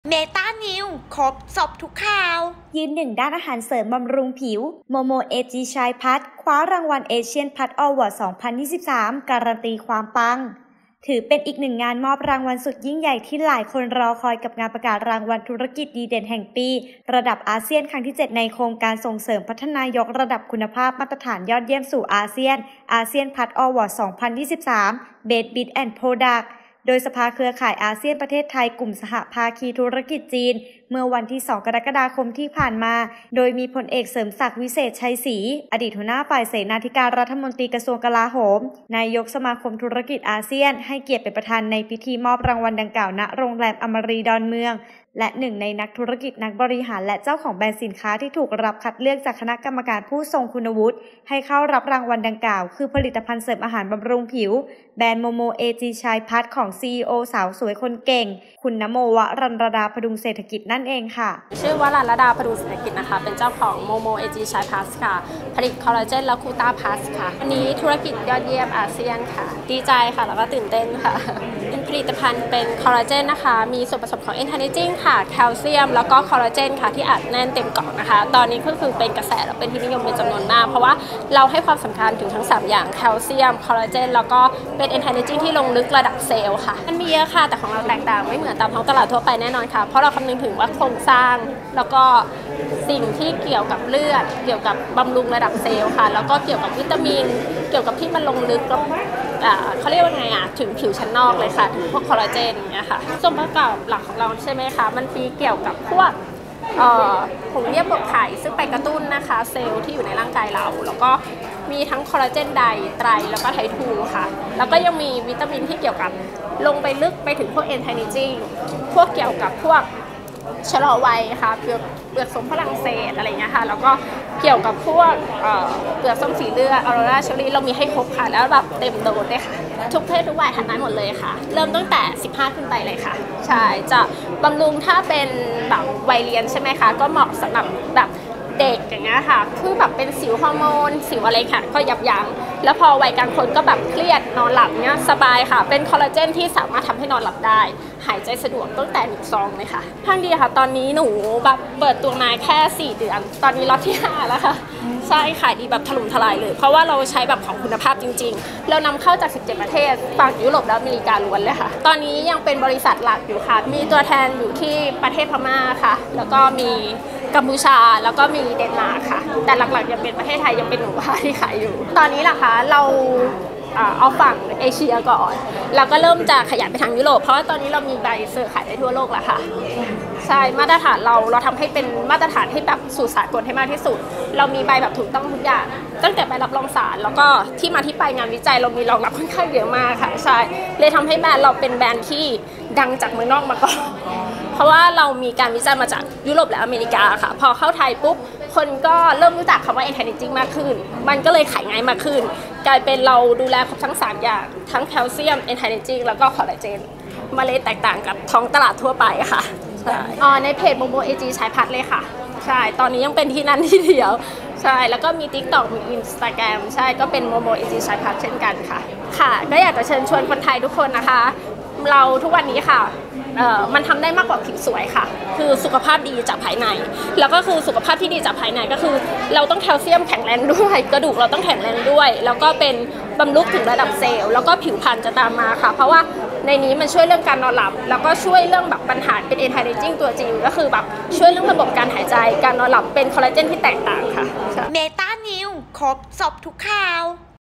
ยืนหนึ่ง! ด้านอาหารเสริมบำรุงผิว Momo ag shine+ คว้ารางวัล ASEAN PLUS AWARDS 2023 การันตีความปัง
ถือเป็นอีกหนึ่งงานมอบรางวัลสุดยิ่งใหญ่แห่งปี ที่หลายคนรอคอย โดยเฉพาะเหล่าบรรดานักธุรกิจ นักบริหาร และเจ้าของแบรนด์ กับงานประกาศรางวัลธุรกิจดีเด่นแห่งปี ระดับอาเซียน ครั้งที่7 ในโครงการ ส่งเสริมพัฒนายกระดับคุณภาพมาตราฐานยอดเยี่ยมสู่อาเซียน ASEAN PLUS AWARDS 2023 BEST BIZ & PRODUCT โดยสภาเครือข่ายอาเซียน-ประเทศไทย กลุ่มสหภาคีธุรกิจจีน จัดขึ้น ไปเมื่อวันที่ 2 ก.ค. ที่ผ่านมา โดยมี พลเอกเสริมศักดิ์ วิเศษไชยศรี อดีตหัวฝ่ายเสนาธิการ รัฐมนตรีกระทรวงกลาโหม นายกสมาคมธุรกิจอาเซียน ให้เกียรติเป็นประธานในพิธีมอบรางวัลดังกล่าว ณ โรงแรม อมารี ดอนเมือง

และหนึ่งในนักธุรกิจ นักบริหาร และเจ้าของแบรนด์สินค้า ที่ถูกรับคัดเลือกจากคณะกรรมการผู้ทรงคุณวุฒิ ให้เข้ารับรางวัลดังกล่าว คือ ผลิตภัณฑ์เสริมอาหารบำรุงผิว แบรนด์ Momo ag shine+ ของ CEO สาวสวยคนเก่ง คุณนะโม วรัญรดา ผดุงเศรษฐกิจ นั่นเอง

และสำหรับผู้ที่สนใจ สามารถสอบถามข้อมูลเพิ่มเติมได้ที่ ag shine+ เพจหลักบริษัท, Namojing, Line : @momoag เบอร์โทรศัพท์